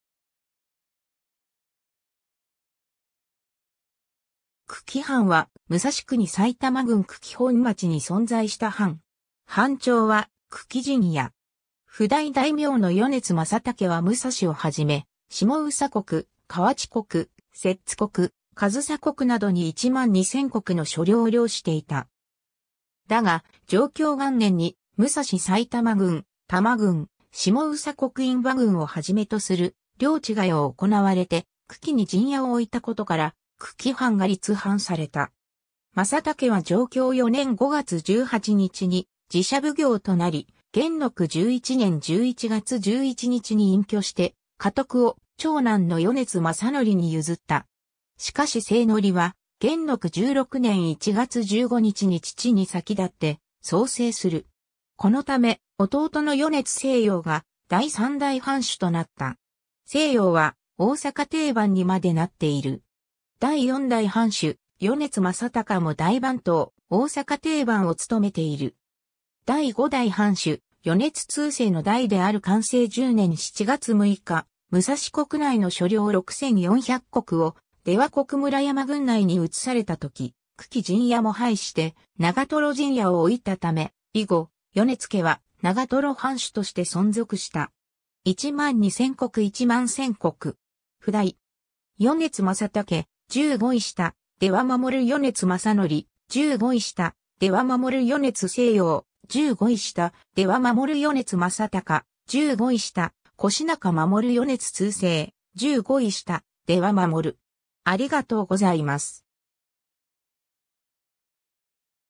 久喜藩
久喜藩（くきはん）は、武蔵国埼玉郡久喜本町（現在の埼玉県久喜市久喜中央および本町、久喜本。古くには久喜本村とも）に存在した藩。
藩庁は久喜陣屋（字荒鎌）。
 譜代大名の米津政武は武蔵をはじめ、下総国・河内国・摂津国・上総国などに1万2000石の所領を領していた。
だが、貞享元年（1684年）に武蔵埼玉郡・多摩郡・下総国印旛郡をはじめとする領地替えを行なわれて、久喜に陣屋を置いたことから、久喜藩が立藩された。
政武は貞享4年（1687年）5月18日に寺社奉行となり、元禄11年（1698年）11月11日に隠居して家督を長男の米津政矩に譲った。
しかし政矩は元禄16年（1703年）1月15日に父に先立って早世する。
このため、弟の米津政容が第3代藩主となった。
政容は大坂定番にまでなっている。
第4代藩主・米津政崇も大番頭・大坂定番を務めている。
第5代藩主・米津通政の代である寛政10年（1798年）7月6日、武蔵国内の所領6400石を出羽国村山郡内に移されたとき、久喜陣屋も廃して長瀞陣屋を置いたため、以後、米津家は長瀞藩主として存続した。
 1万2000石→1万1000石。
譜代。
 米津政武（まさたけ）〈従五位下・出羽守〉 米津政矩（まさのり）〈従五位下・出羽守〉 米津政容（まさよし）〈従五位下・出羽守〉 米津政崇（まさたか）〈従五位下・越中守〉 米津通政（みちまさ）〈従五位下・出羽守〉